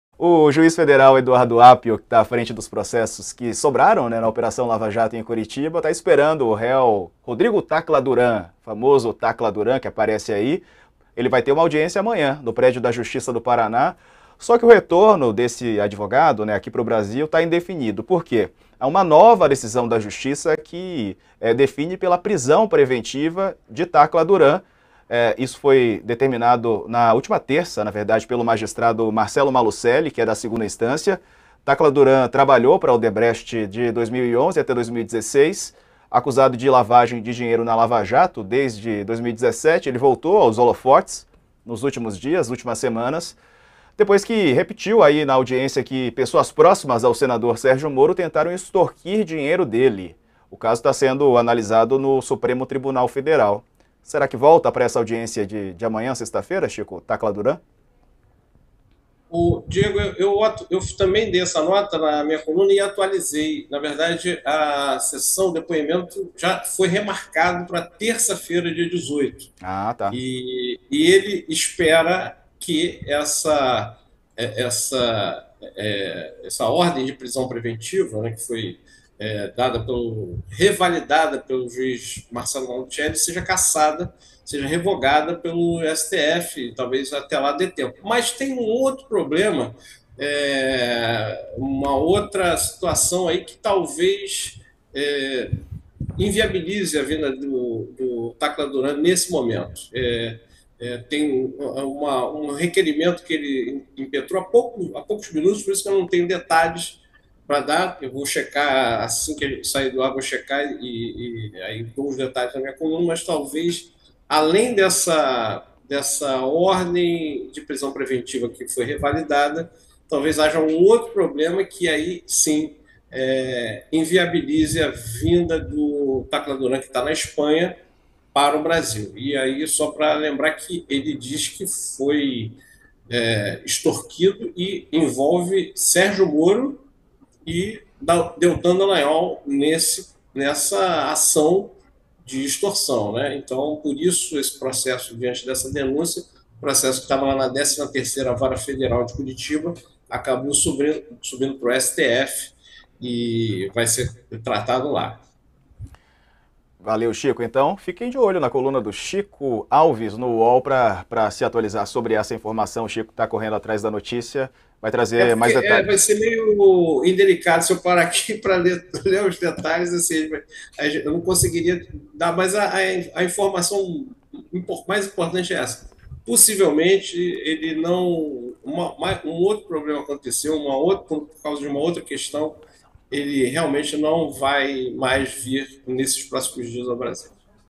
Desafeto de Moro, Tacla Duran vem ao Brasil para depor ao juiz da Lava Jato | Chico Alves
Reportagem do colunista Chico Alves mostra que o advogado Rodrigo Tacla Duran deverá desembarcar nos próximos dias no Brasil, vindo da Espanha, onde mora, para depor ao juiz Eduardo Appio, da 13ª Vara Federal de Curitiba (PR), que trata dos casos relativos à Operação Lava Jato. O depoimento está marcado para terça-feira (18), às 16h.